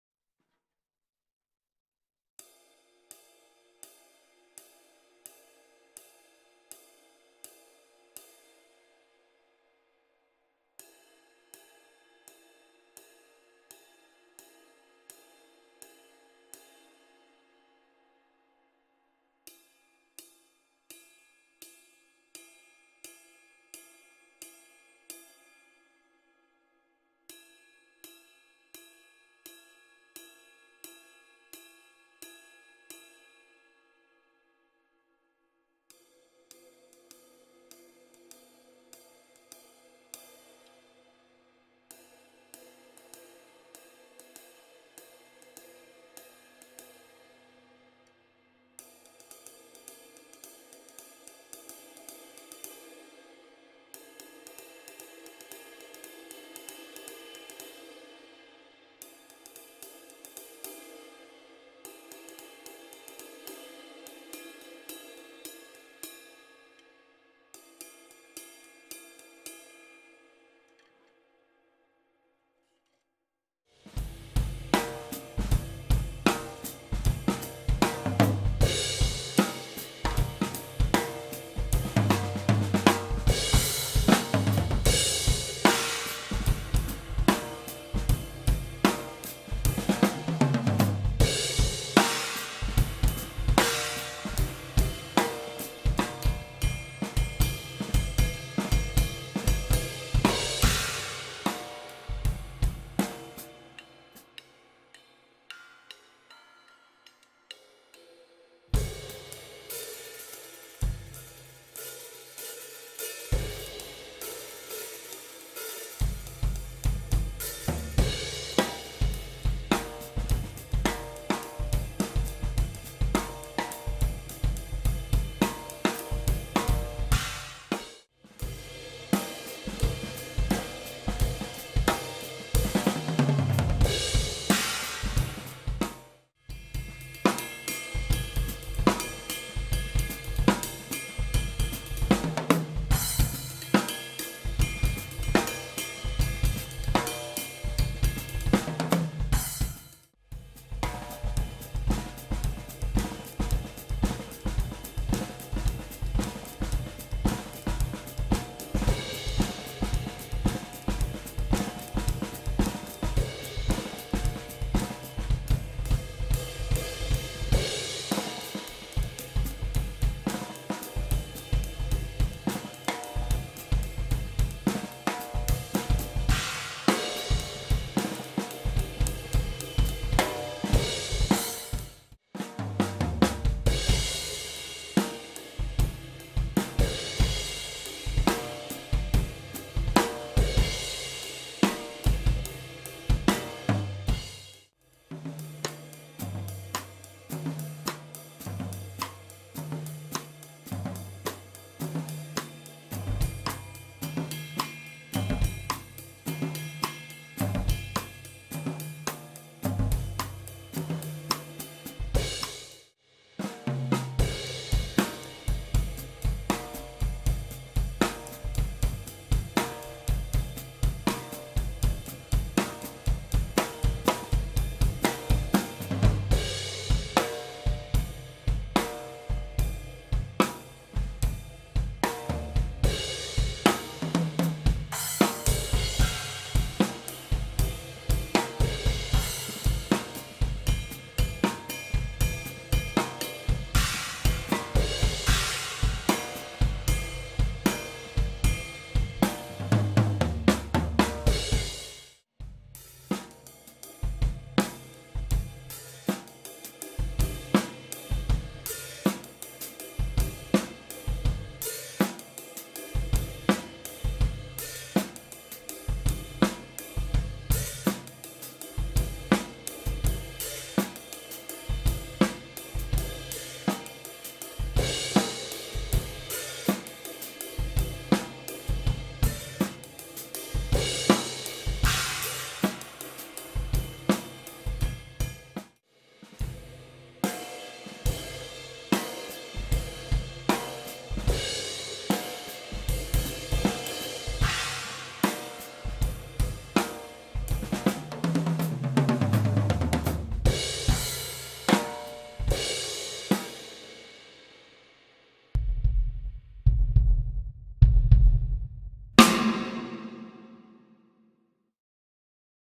Zildjian K Light Ride 22" Demo (2514 g)
This video features my new ride cymbal - a Zildjian K Light Ride in 22" with the weight of 2514 g.

Other cymbals:
Zildjian K Custom Dark HiHat 14"
Paiste Signature Full Crash 16"
Paiste Sound Formula Reflector Splash 10"
Paiste Signature Dark Energy Crash Mk I 18"
Paiste Signature Thin China 16"

Drums:
Pearl Masters Premium Maple MMP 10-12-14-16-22
DW Collectors Exotic VLT 14" x 6.5"

Audio:
Zoom H5 internal X/Y-microphone in overhead position
Snare: Shure SM57
Bassdrum: Shure Beta 52a

No EQ, no FX, no Dynamics on the audio tracks!